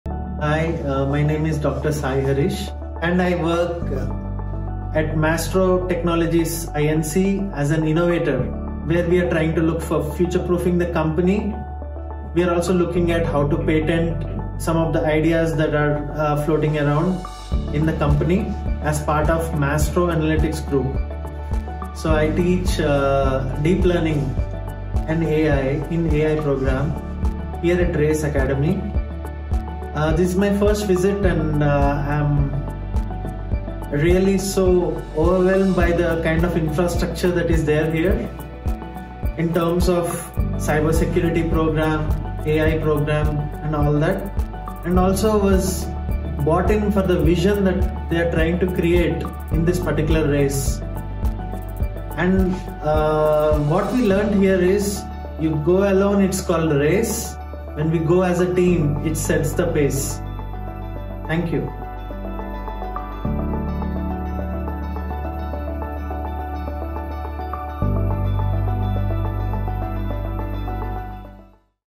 Dr. Sai Hareesh Endorsing RACE, REVA University 2023 | Deep Learning | Artificial Intelligence - AI
Dr. Sai Hareesh is an active public speaker, currently working as an Innovator at Maestro Technologies and mentoring potential aspirants in RACE, REVAUniversity. He has completed his Ph.D. in Computer Vision from Sri Sathya Sai Institute of Higher Learning. He has 12+ years of working experience in both Academia and Industry. He works on various AI (ML + DL + MR) algorithms for HR, Defense, Healthcare, Telecom, Industrial IOT, Digital Marketing, Retail, Business and Renewables domain data.

He has been introduced to an assorted 28 technical talks and seminars. He has shown excellence with counting more than 35 publications, 55 patents/disclosures, and 35 recognised awards.

He has Hands-on experience in Computer Vision (video, image), NLP(text), IoT (time-series, numerical data ), and Speech (Audio) complemented with Data Visualization and Creative Ideation.